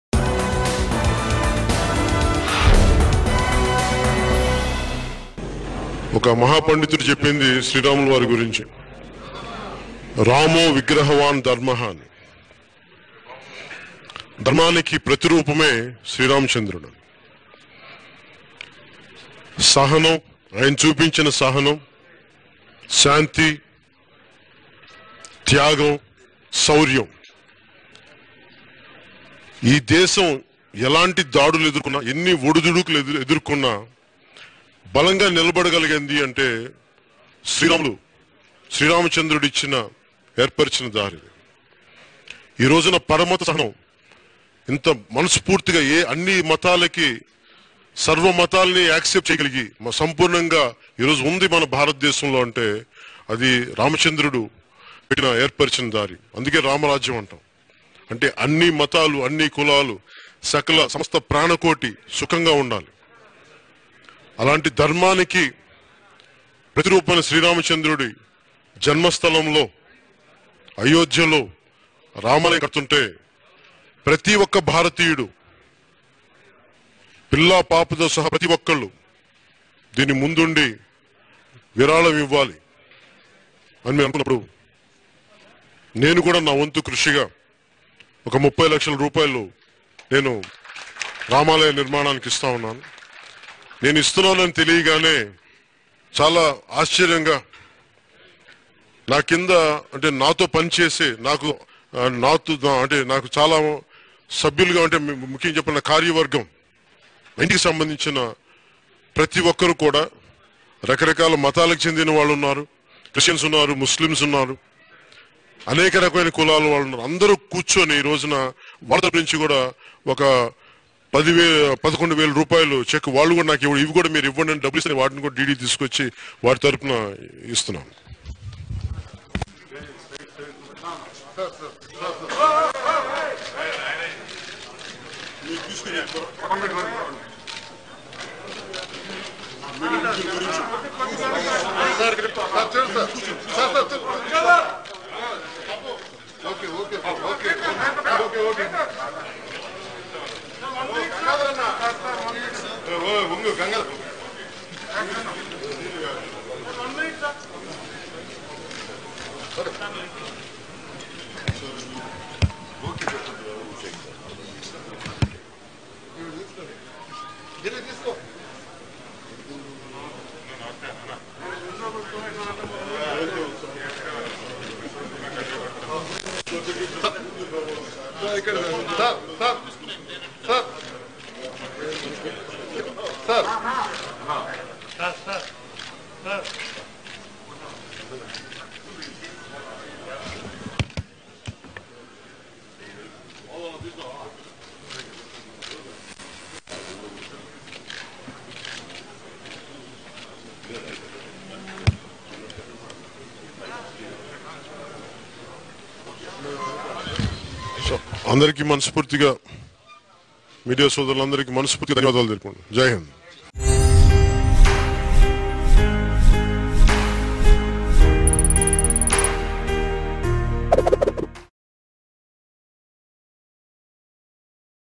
Janasena Chief Pawan Kalyan Donates Rs 30 Lakhs For Ram Temple In Ayodhya | AP News | Mango News
Janasena Chief Pawan Kalyan Donates Rs 30 Lakhs For Ram Temple In Ayodhya.